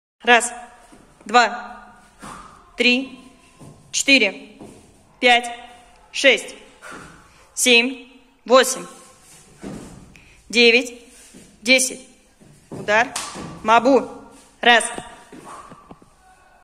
8 июня,2022
Сёма на тренировке по Ушу-Тауло